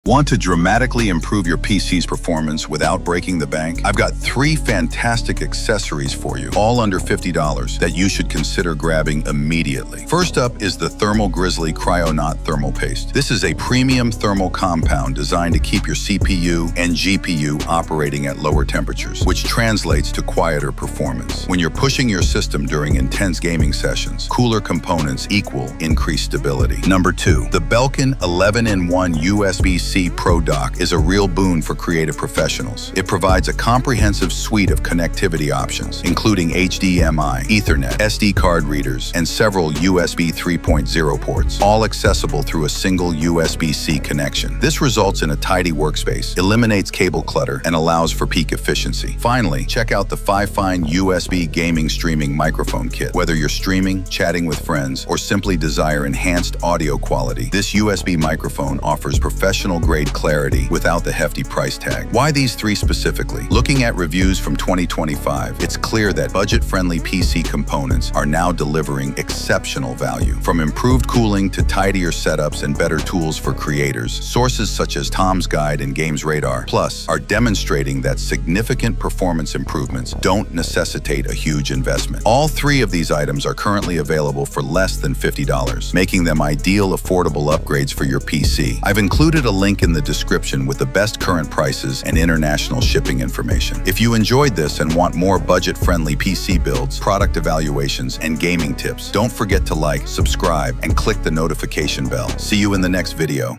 3 MUST-HAVE PC Accessories Under $50 You’ll Actually Use! 💻🔥
Looking to elevate your PC experience without emptying your wallet? Here's the inside scoop on the three hottest, most dependable PC accessories you can snag for under $50 in 2025. We've rigorously tested them, we trust their performance, and they're absolutely taking the tech world by storm. Get ready to enhance your setup!

🎯 Gear featured:
1️⃣ Thermal Grizzly Kryonaut Thermal Paste
2️⃣ Belkin 11-in-1 USB-C Pro Dock
3️⃣ FIFINE K690 USB Microphone

📦 Grab the best deals from the links below & upgrade smart!